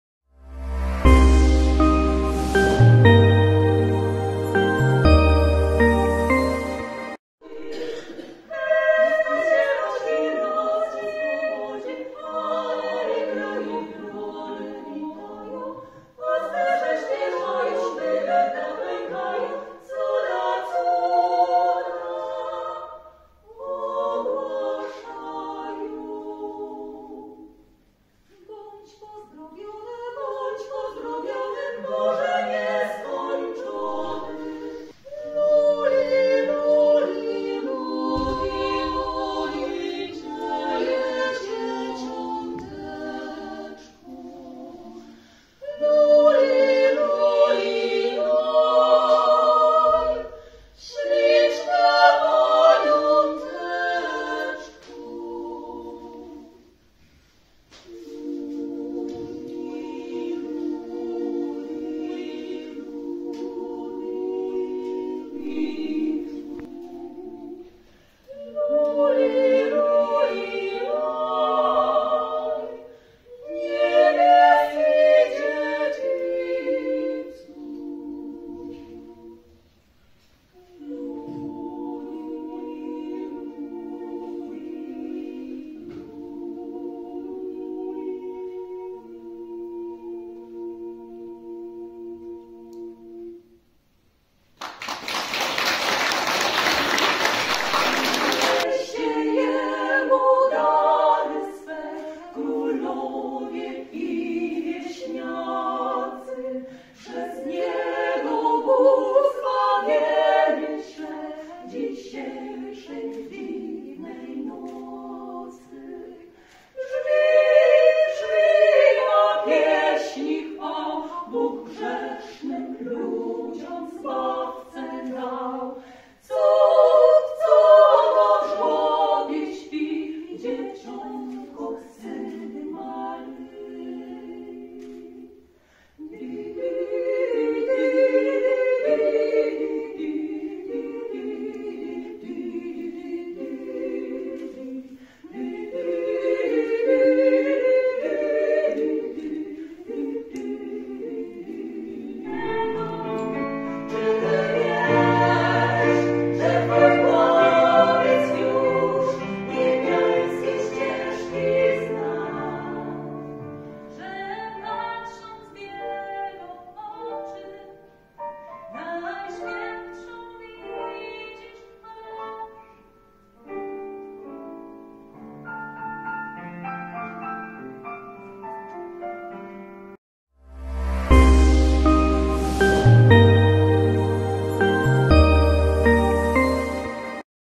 TRZECIE ZWROTKI - piękne kolędy w Klubie Oczko
Trzecie Zwrotki to zespół , który tworzą: Joanna Górska, Bożena Harasimowicz-Pęza, Izabela Kiernozek, Dorota Rypina-Gałązka oraz Ewa Szelkowska-Woźniczko. W Klubie Oczko w Ostrołęce panie wystąpiły w koncercie kolęd.